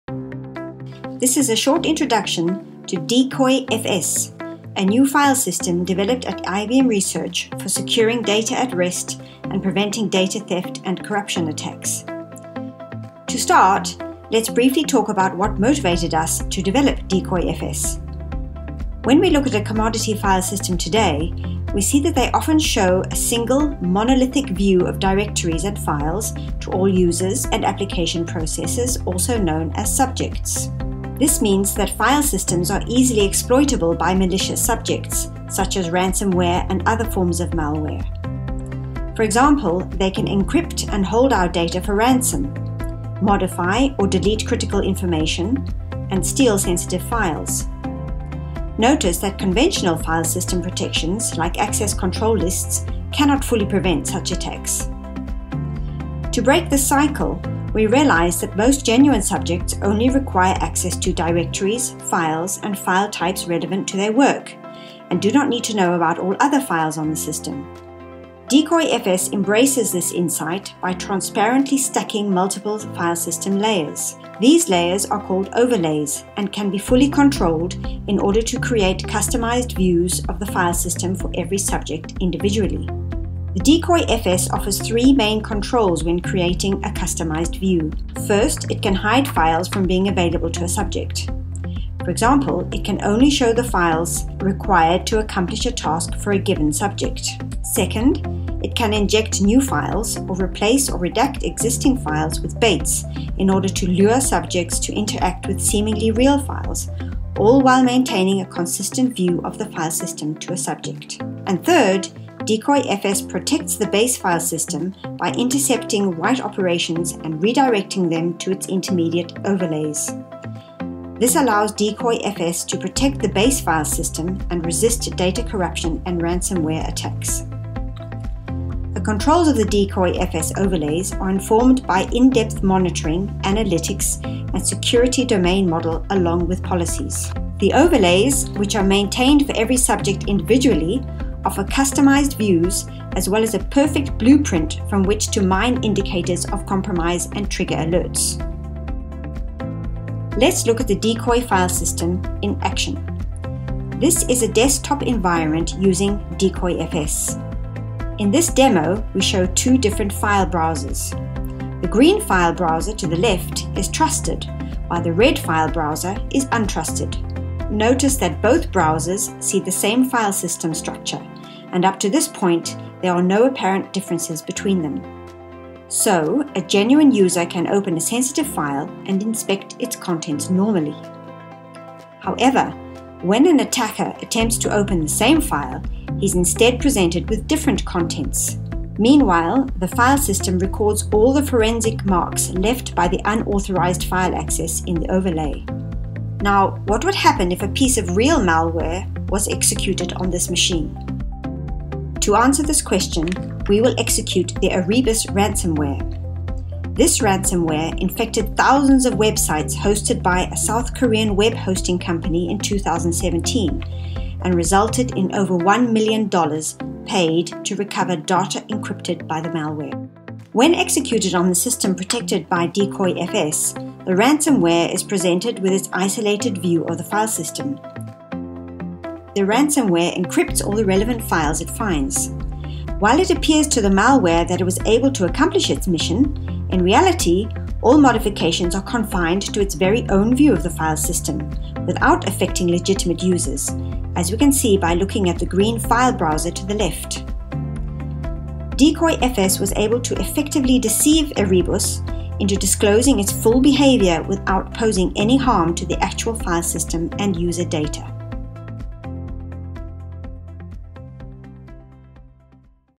IBM Research: Decoy FS
A short introduction to DcyFS, a new filesystem developed at IBM Research for securing data at rest and preventing data theft and corruption attacks